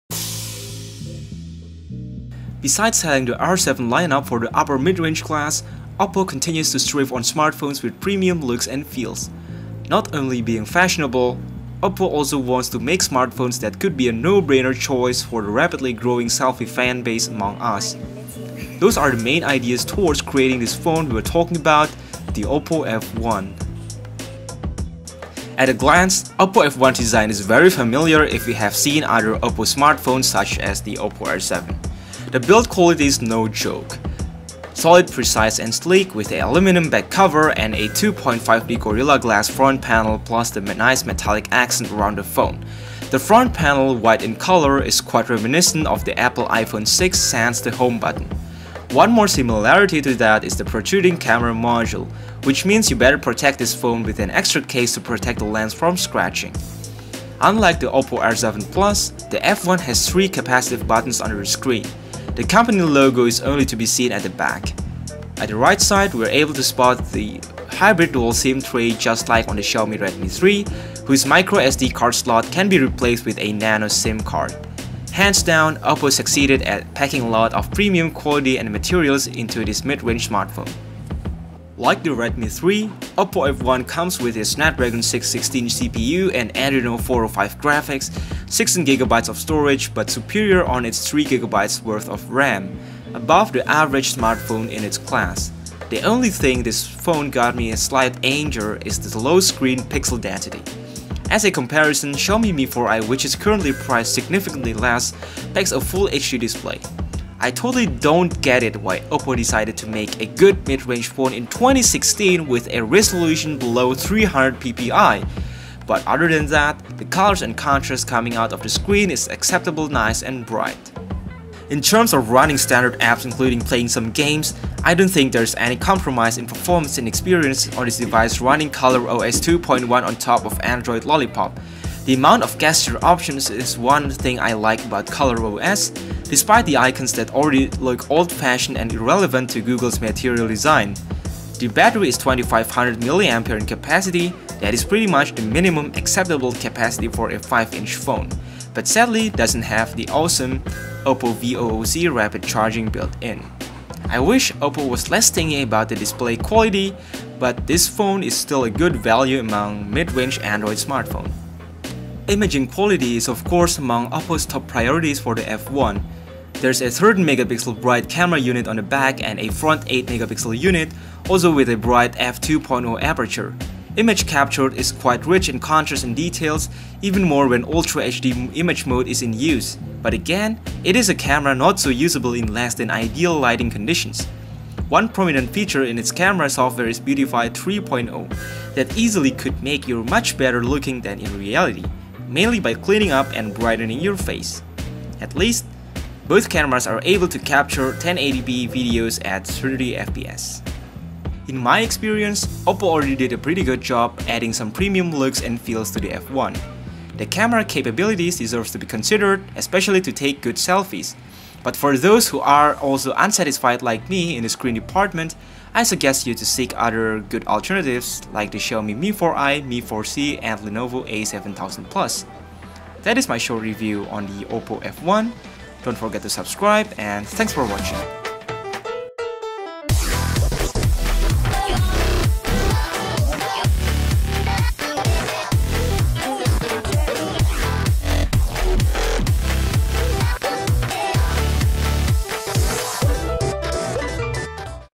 Oppo F1 Review | English
Besides the R7 lineup, Oppo F1 was made for a more modest segment but still with quality and premium design in mind. Watch our review till the end!

Spec :
CPU : Octa-core 1.7 GHz Cortex-A53
GPU : Adreno 405
Internal : 16 GB, 3 GB RAM
Camera : 13 MP, Aperture f/2.2, phase detection autofocus, LED flash, 8 MP, Aperture f/2.0, 1080p, screen flash
Card Slot : microSD, up to 128 GB (uses SIM 2 slot)
OS : Android OS, v5.1 (Lollipop)
Res : 1080 x 1920 pixels (~294 ppi pixel density)

Special Thanks:
Zahrina Indah
Masatommi M.